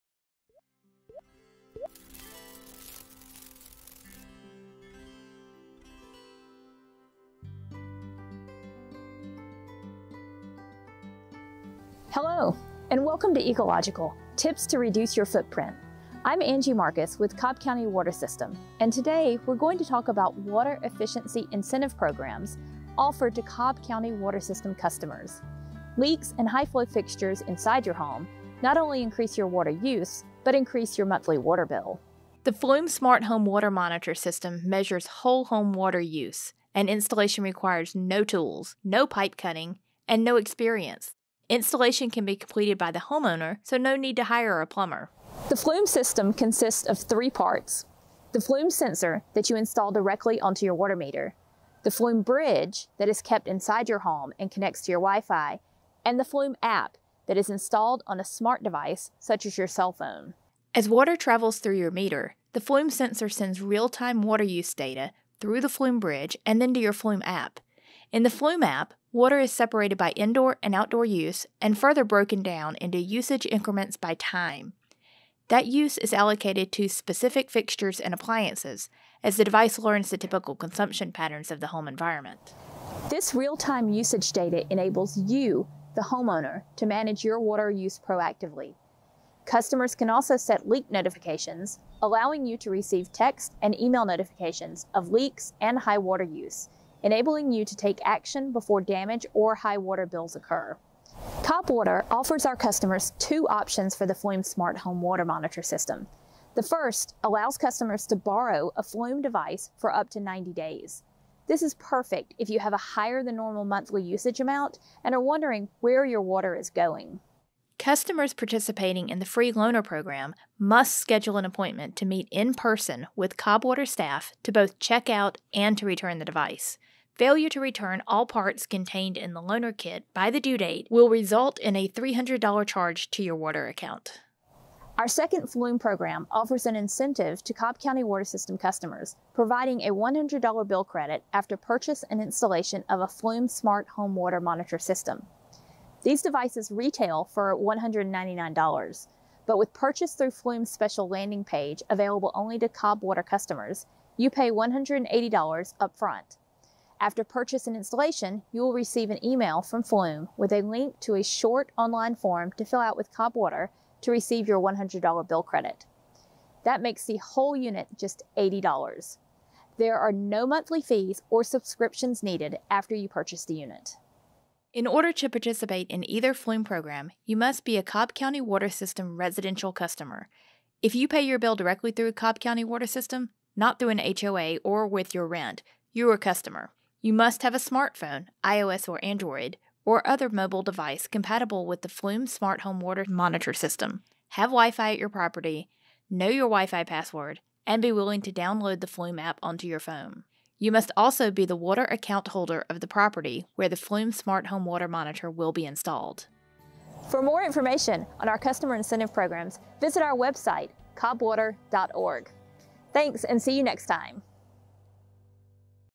Eco Logical Flume Home Water Monitor - March 07, 2023
Cobb County Water System tells us how we can monitor our water usage and get real-time data on how much water is being used at any given time!